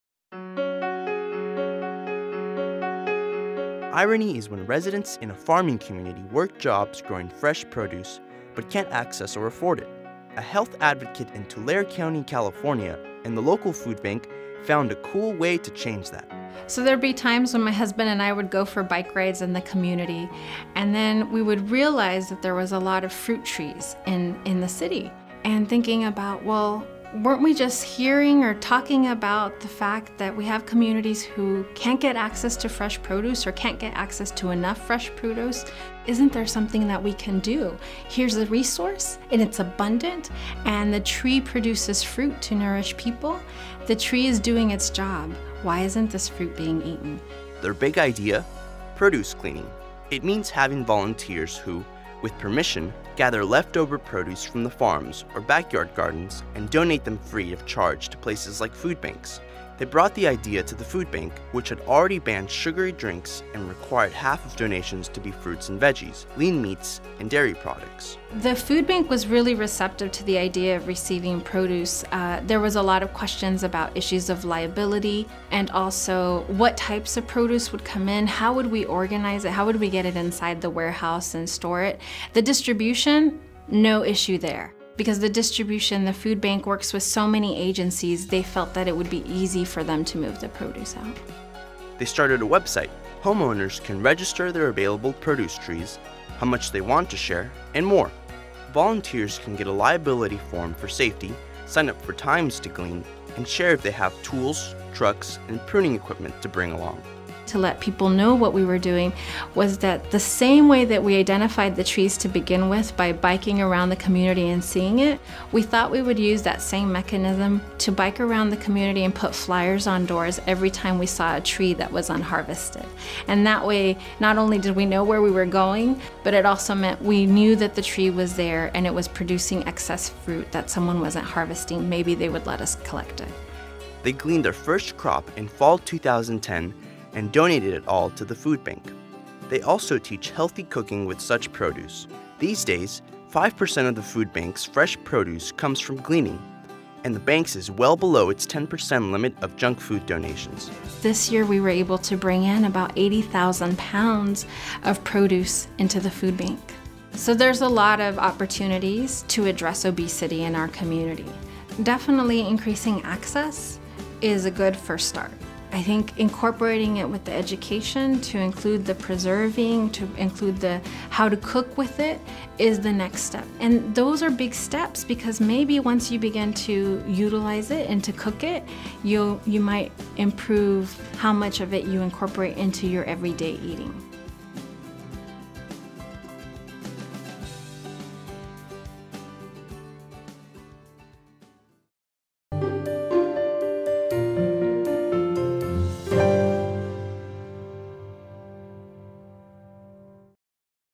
Tree shaking and fruit moving for nutrition
Latinos make up over 60% of residents in Tulare County, some of these families aren’t getting proper nutrition, leading to diet-related conditions, like obesity and diabetes. When the local food bank revamped their nutrition policy to encourage more fresh produce donations, a local health advocate used her hard-earned knowledge and passion to support that policy.